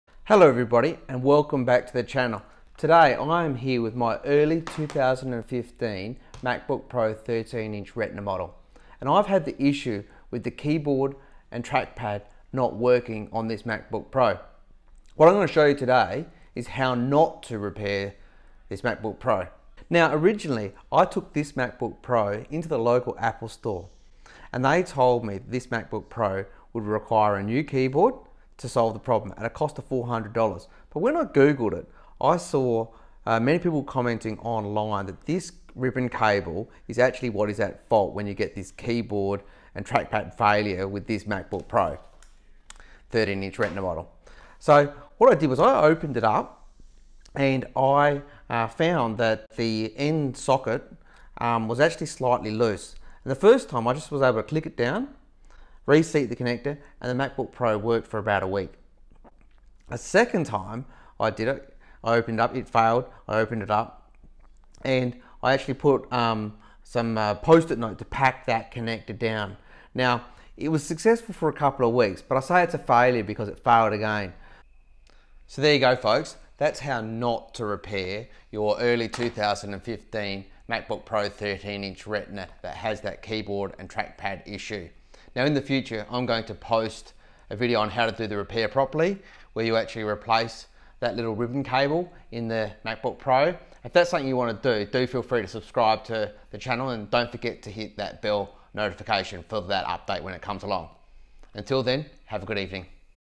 2015 Apple Macbook Pro 13" Retina A1502 - Keyboard and Trackpad Issue - How NOT to fix!!
In this video I show how I attempted to fix the common Keyboard and Trackpad Issue on my Early 2015 Macbook Pro 13" Retina (A1502).

The fix only lasted a few weeks, thus I declare it a fail!

I have had made this video for 2 reasons:

1/ If you have the same issue you can attempt this fix before replacement parts come in the post; and
2/ To simply show you not what to bother trying as whilst successful initially it only lasted a few weeks!

The problem actually is the Trackpad cable and you can purchase a non-OEM cable here:

Amazon (US and General)
Amazon (Australia)

You can purchase the tools required for this task here:

Amazon (Australia)

Here is a link to a video I did of actually replacing the faulty Trackpad cable:

I trust you find this video useful!